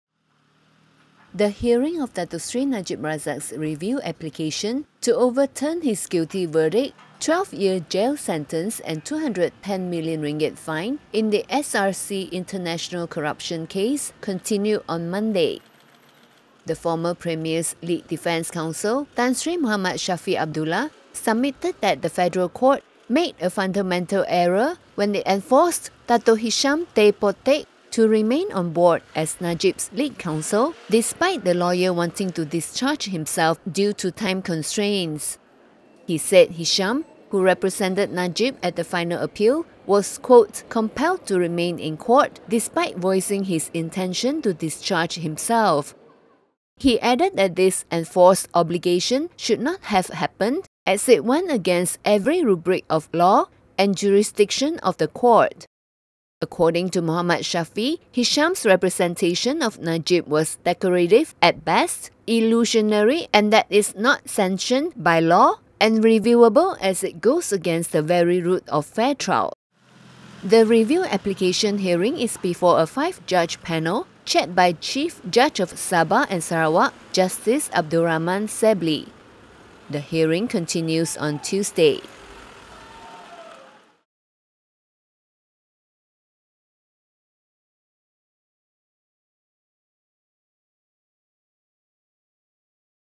Lawyer forced to stay on as Najib's counsel was a 'fundamental error' by court, says Shafee
The Federal Court made a "fundamental error" when it enforced that Datuk Hisyam Teh Poh Teik to remain on board as Datuk Seri Najib Razak's lead counsel despite the lawyer wanting to discharge himself due to time constraints, says Tan Sri Muhammad Shafee Abdullah.